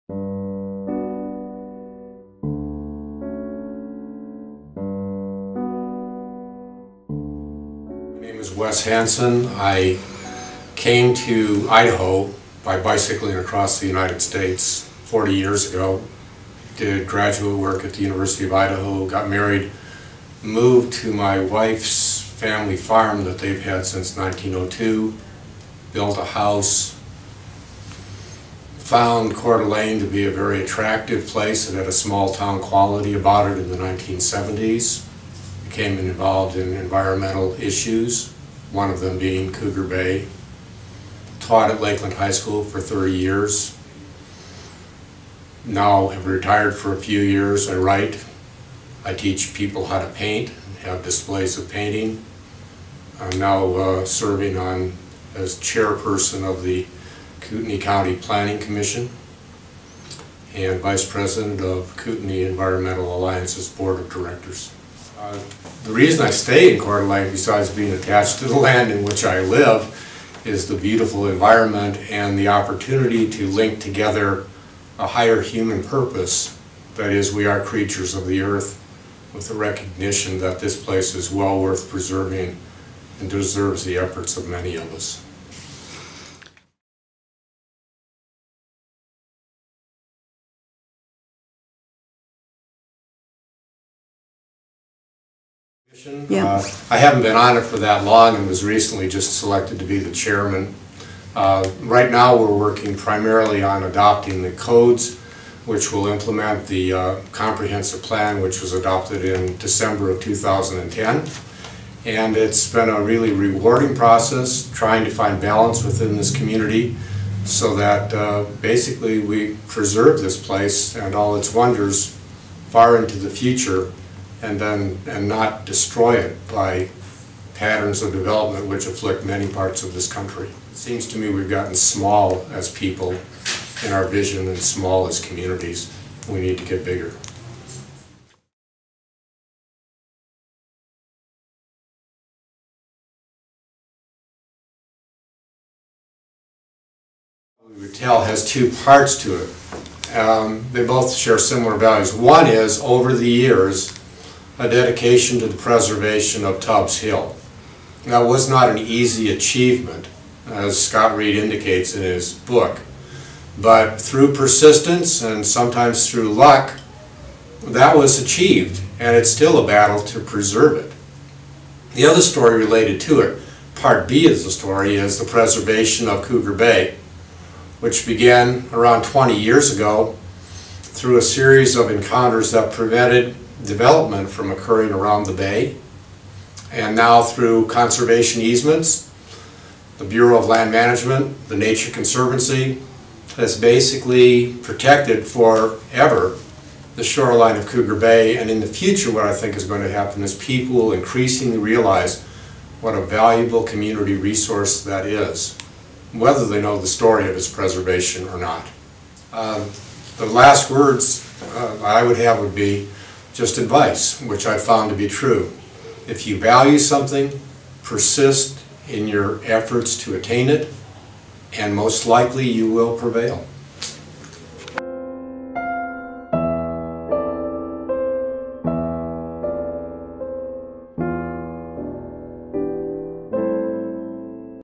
Wes Hanson_Protecting the Environment We Share
Wes Hanson lives his life in a way that is aligned with the words that he speaks.  He said during the longer interview that this place is well worth preserving and deserves the efforts of all of us.  His consistent efforts have supported that thought.